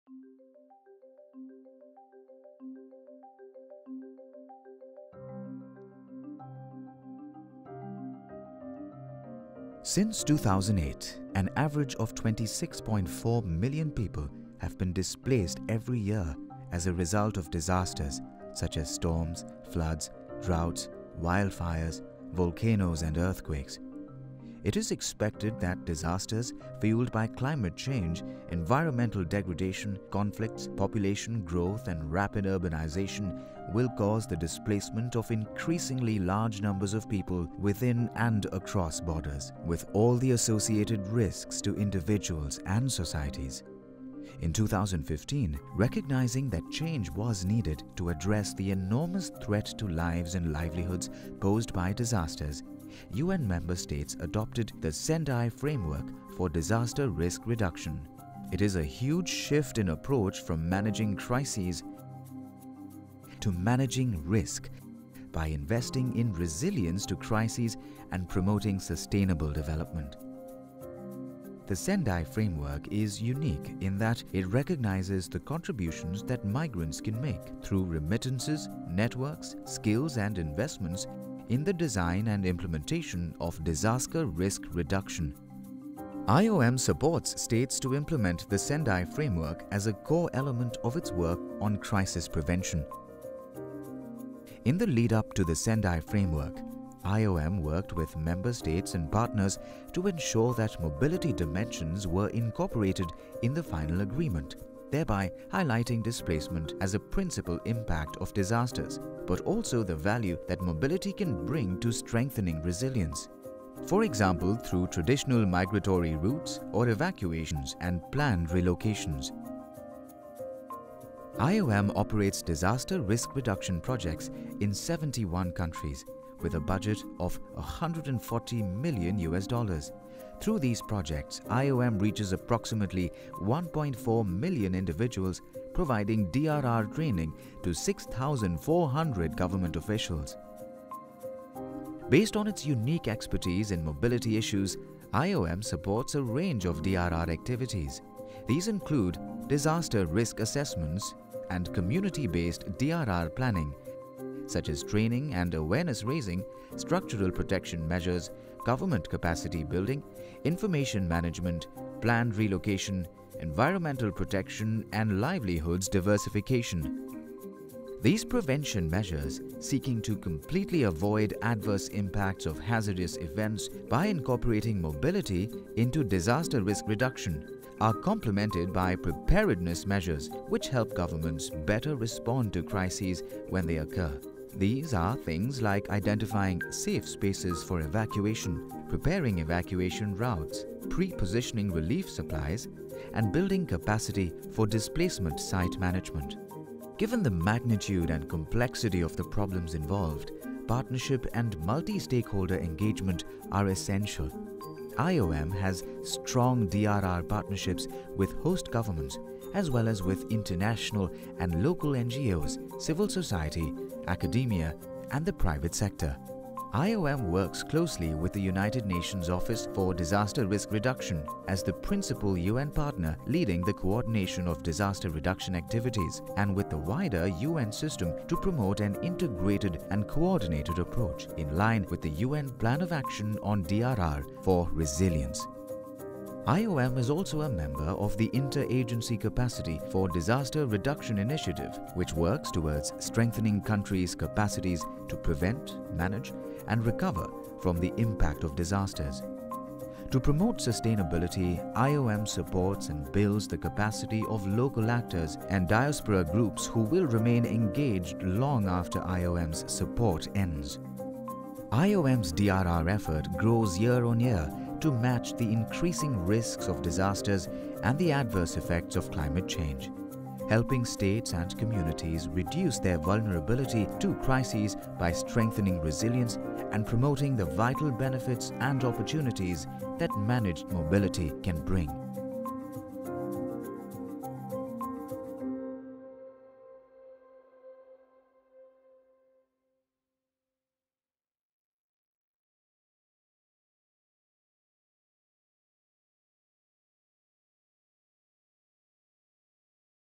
Building Resilience with Disaster Risk Reduction
Disasters fueled by climate change, environmental degradation, conflicts, population growth and rapid urbanization will lead to rises in displacement, both internally and across borders. The International Organization for Migration (IOM) supports states to implement the Sendai Framework for Disaster Risk Reduction adopted in 2015 by UN member states.